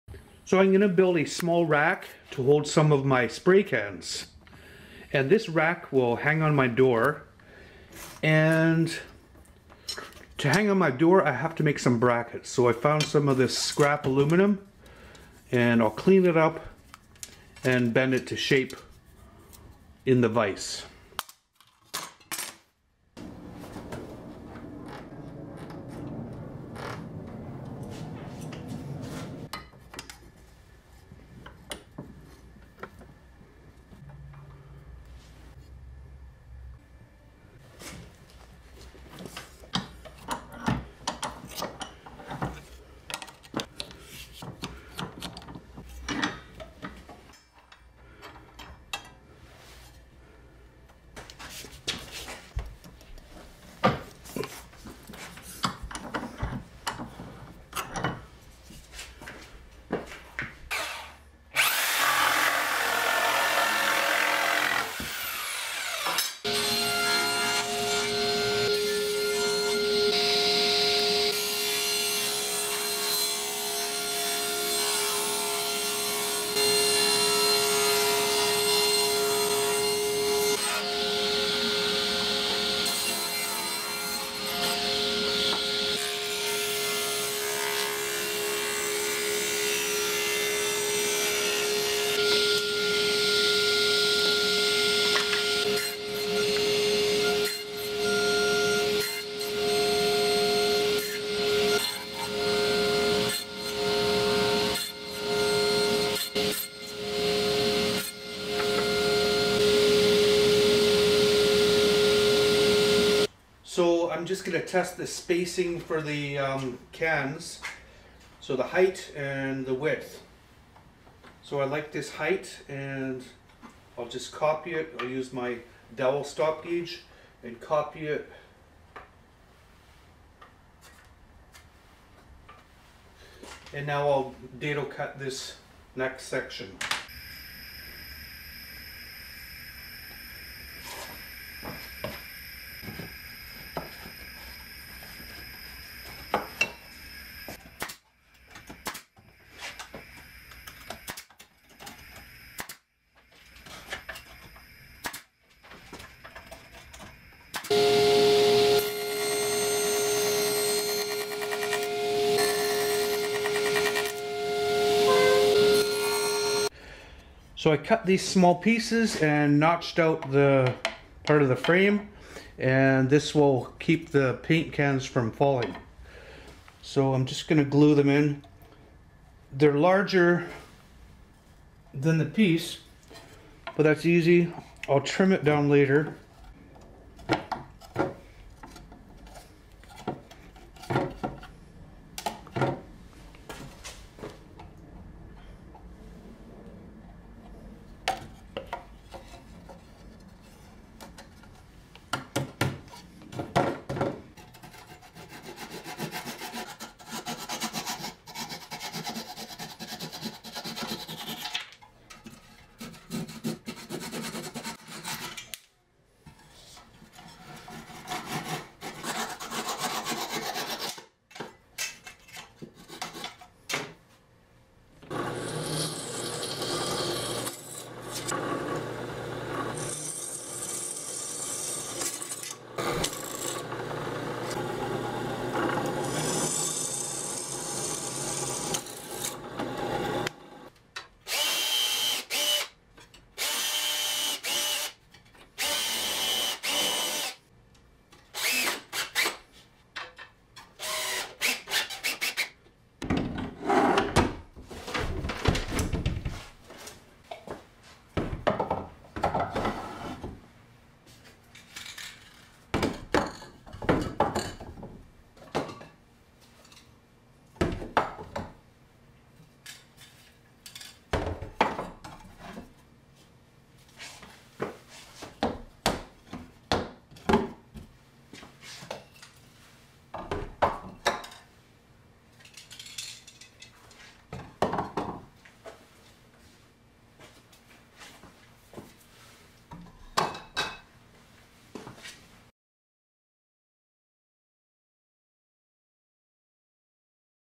Spray can door rack
Easy to make spray can rack that hangs on a door. Looks neat and tidy storing up to 18 cans, the ones I use the most.